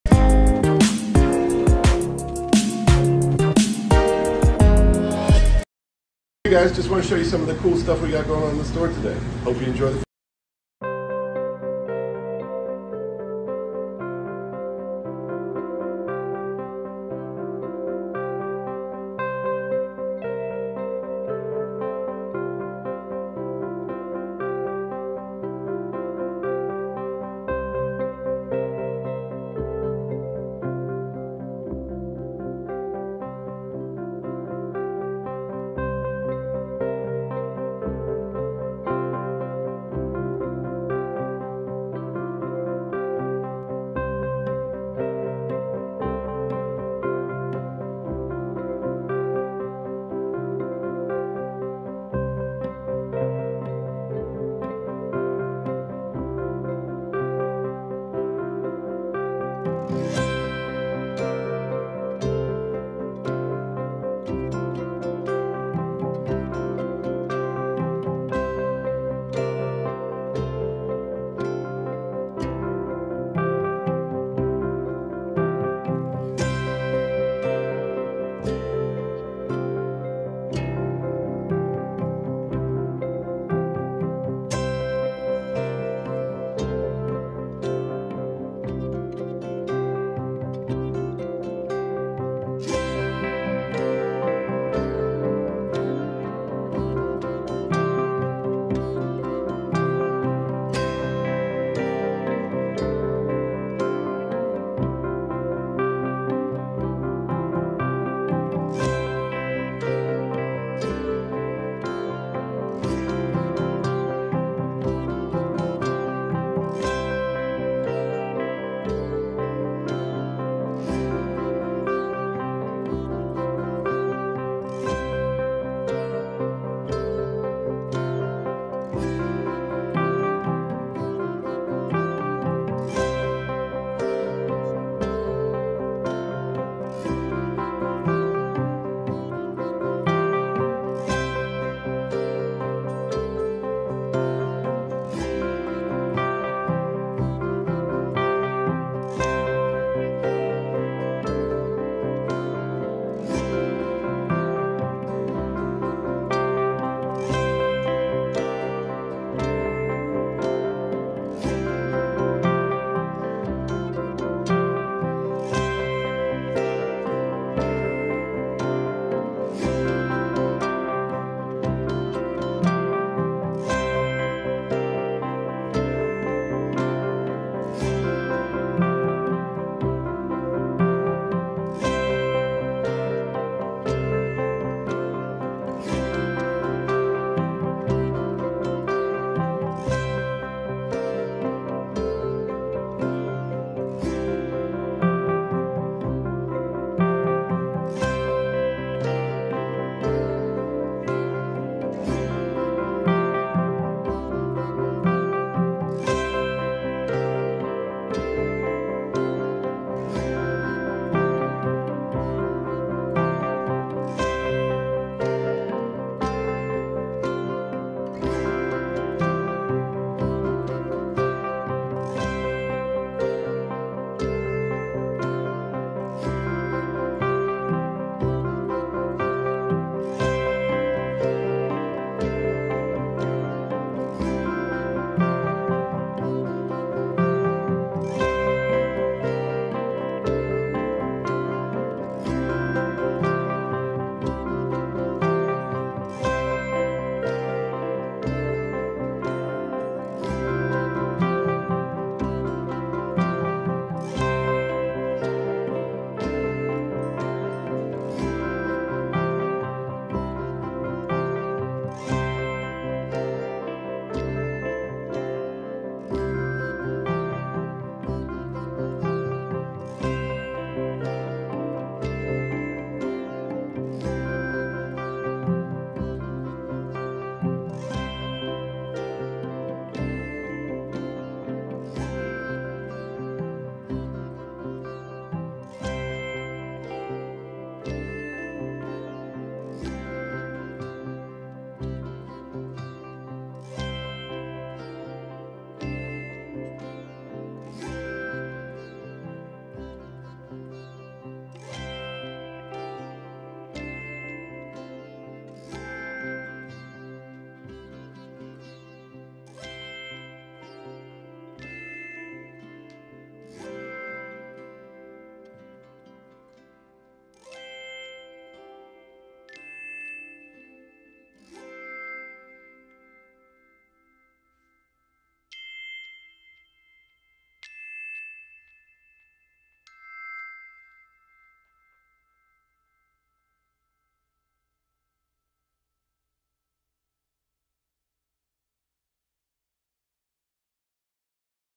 Fish store tour - Freshwater section @ somethingFISHY
We wanted to show you all some of the fish we have in at the store this week.  We have way more fish than this, so stop on by.